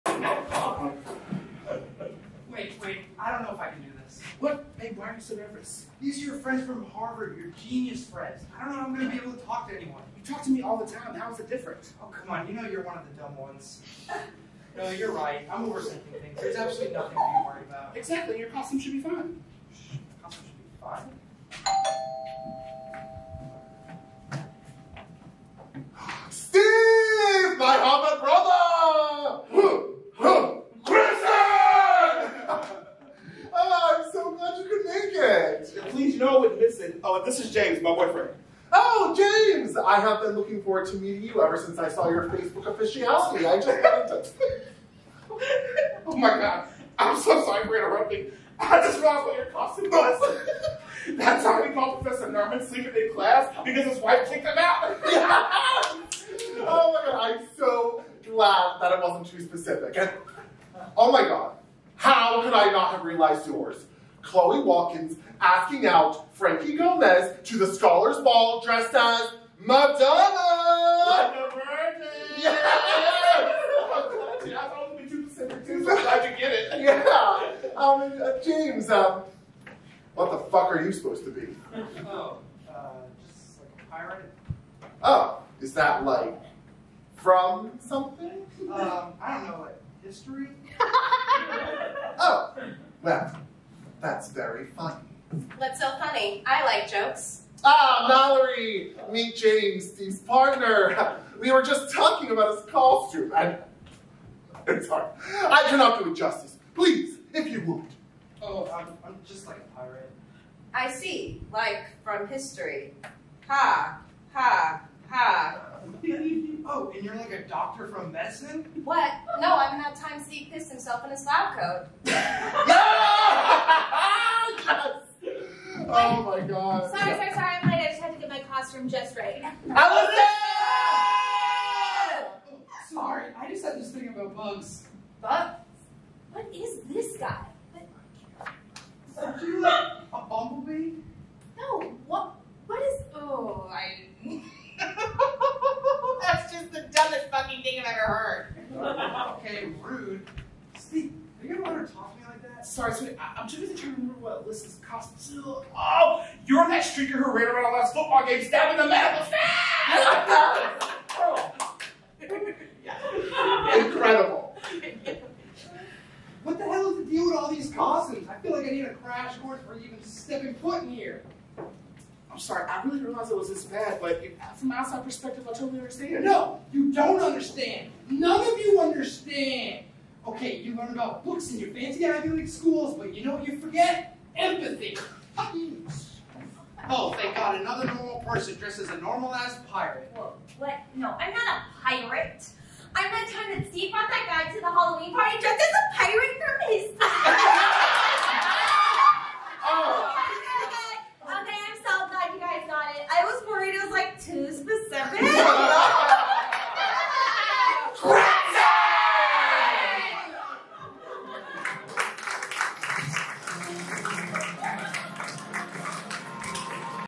Specific Costumes by Marshall B Mulkey | Triangle Sketch Comedy
Featuring: Phil Centore, Desmond Leach, Isabella Brezenski, Sierra Smith, Dan Cerullo
Written by: Marshall B Mulkey
Directed by: Mitchell Aaron Mulkey

Performed at TR Studio

About the Show: Triangle Sketch Comedy Presents The Uproar - A Hellishly Funny Sketch Show!
You’ll be laughing ‘til you’re coffin, uh we mean coughing. With 90 minutes of original sketches, live performances, videos, and music written and performed by local artists in the Triangle, this Halloween show is sure to be eerie-sistible. Performed in October 2023.